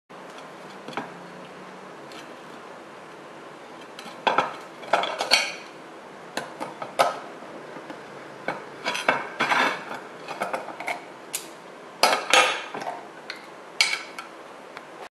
Tramontina Can Opener
Opens tight bottles caps and closes them tightly with ease.